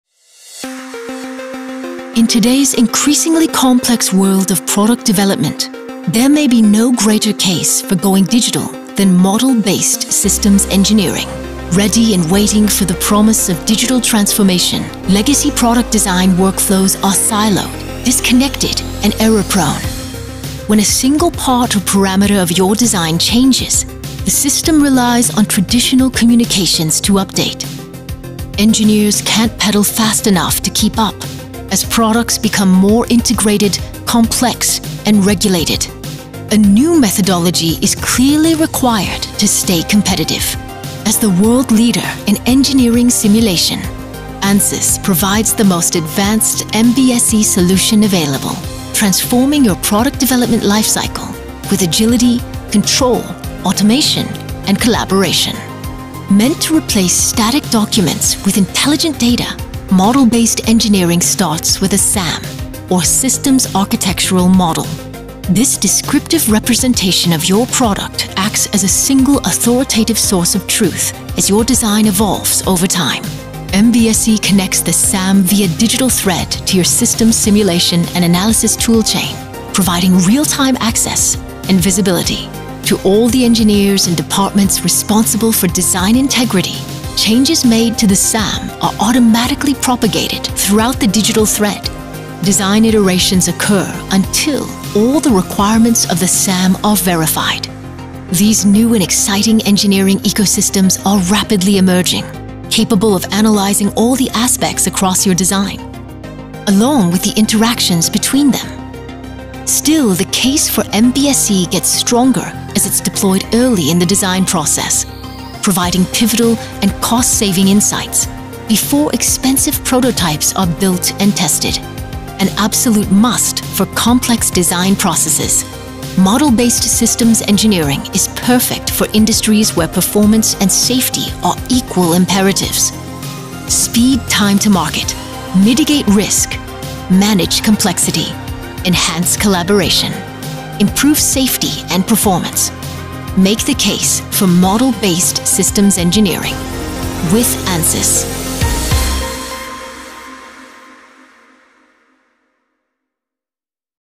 Revolutionizing Product Design with Model-Based Systems Engineering (MBSE)
The future of product development is here, and it starts with Model-Based Systems Engineering (MBSE). Traditional workflows are siloed, disconnected, and prone to errors—making it harder than ever for engineers to keep up with today’s complex, integrated, and regulated product designs.

In this video, we dive into how Ansys, the world leader in engineering simulation, transforms product development through MBSE. By replacing static documents with intelligent data and utilizing Systems Architectural Models (S.A.M.), Ansys' MBSE solution provides:
- Real-time collaboration and visibility across engineering teams.
- Automated design updates via a seamless digital thread.
- Cost-saving insights early in the design process, minimizing the need for expensive prototypes.

Perfect for industries prioritizing performance and safety, MBSE accelerates time-to-market, enhances collaboration, and ensures design integrity like never before.

Discover how Ansys is driving the digital transformation of engineering with agility, control, and innovation. Watch now to see how MBSE can elevate your product development process.

00:00 The Case for Model-Based Systems Engineering (MBSE)
00:31 Why a New Methodology is Needed
00:44 Ansys: Transforming Product Development with MBSE
00:57 Systems Architectural Models (S.A.M.): A Single Source of Truth
01:16 Real-Time Collaboration with the Digital Thread
01:49 Benefits of MBSE in Early Design Stages
02:06 Industries That Benefit from MBSE